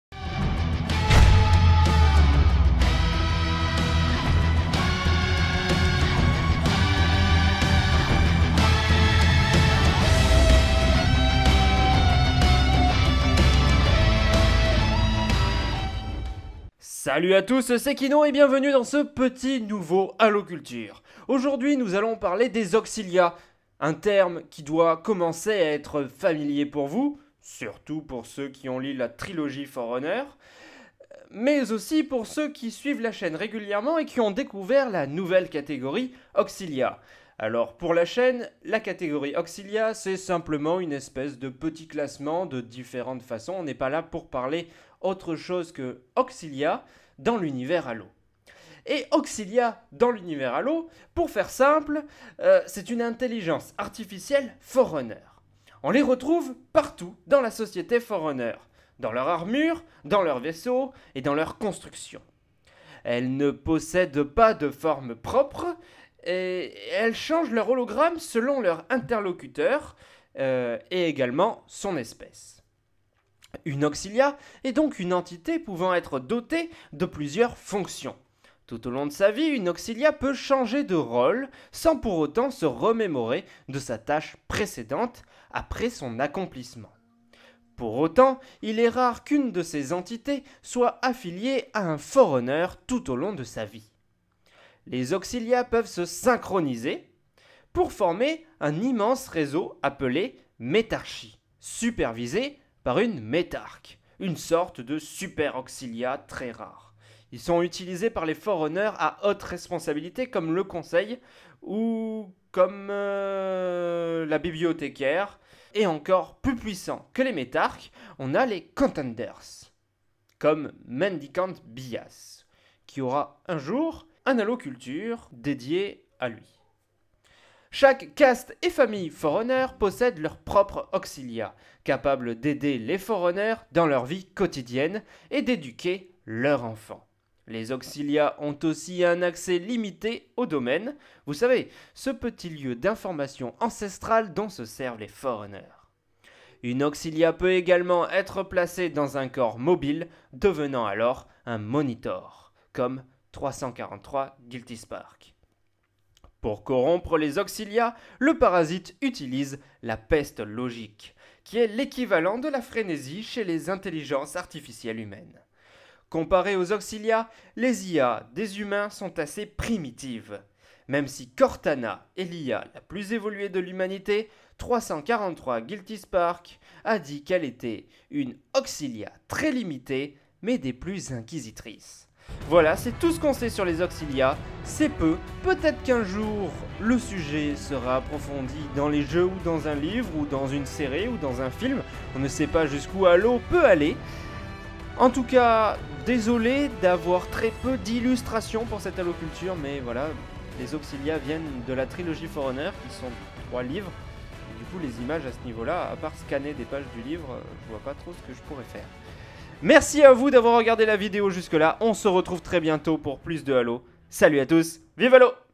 Halo Culture - Auxilia
Les Forerunners avaient beaucoup recours aux Auxilias, voyons avec Kino ce qu'il y a à savoir sur eux !
Pour plus d'explication encore, et tout connaitre de l'histoire d'Halo,